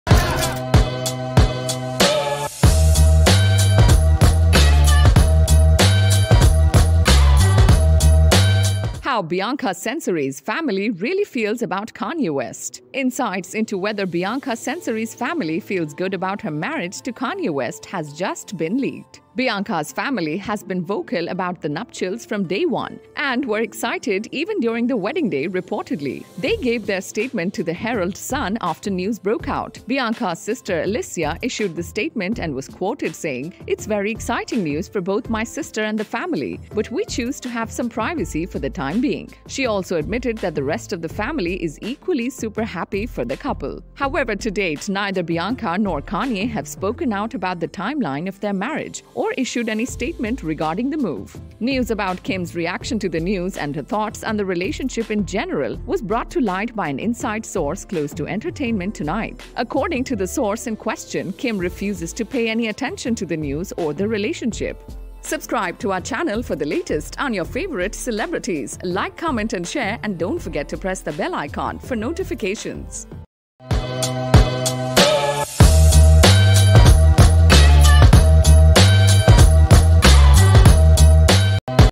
How Bianca Censori’s family ‘really’ feels about Kanye West
Bianca Censori’s family feels good about her marriage to Kanye West has just been leaked